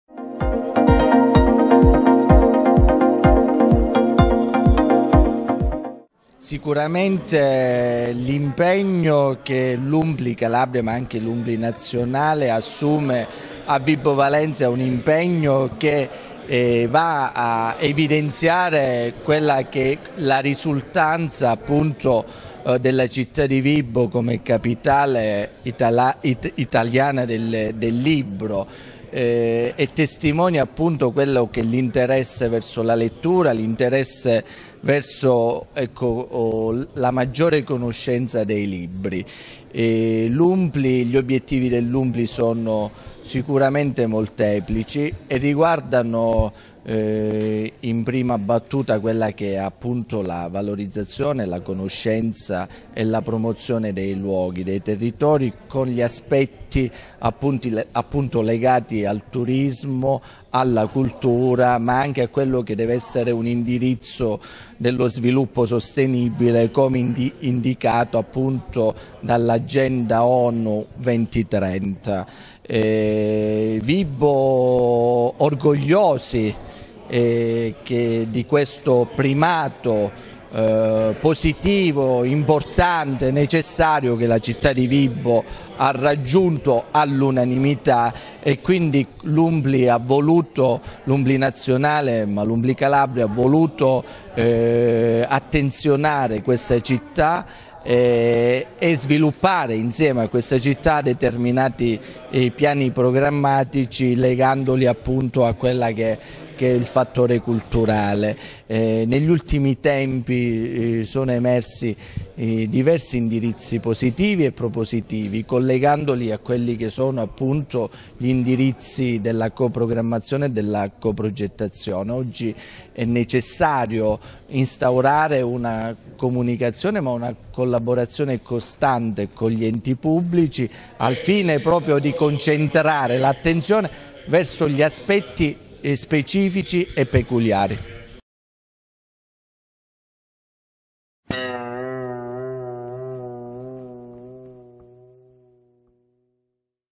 Antonello Grosso La Valle - Presentazione di "Pro Loco, risorsa per il territorio"
Antonello Grosso La Valle, Presidente UNPLI Cosenza - Presentazione di "Pro Loco, risorsa per il territorio"
Registrazione a cura di Antonio Abbruzzese effettuata il 27 giugno 2021 presso il Palazzo Comunale di Vibo Valentia